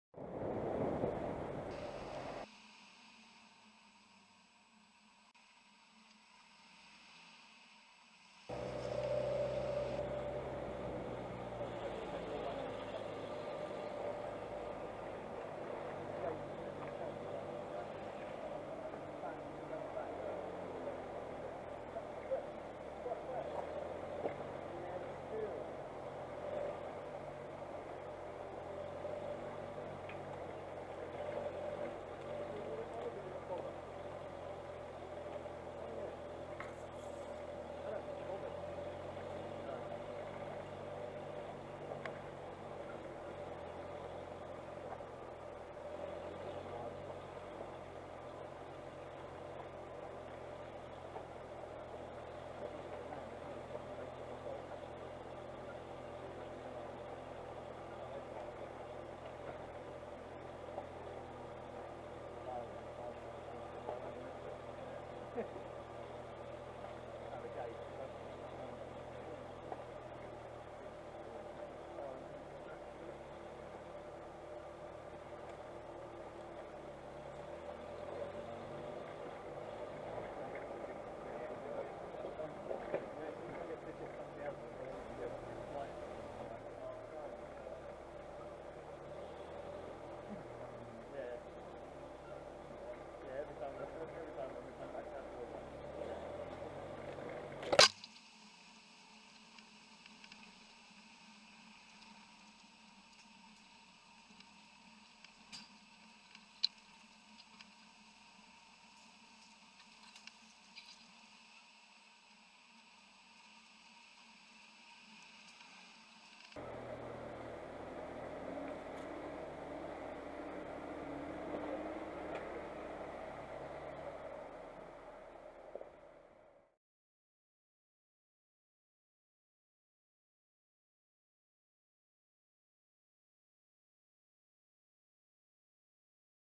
Plug Lane - Kent Byways
Plug Lane is a byway between Meopham & Vigo. It starts off paved as part of a driveway and then turns into dirt/stone. Real easy one this, suitable for any 4x4.

Starts on Dean Lane
Ends on Leywood Road

Air Fresheners now in stock!!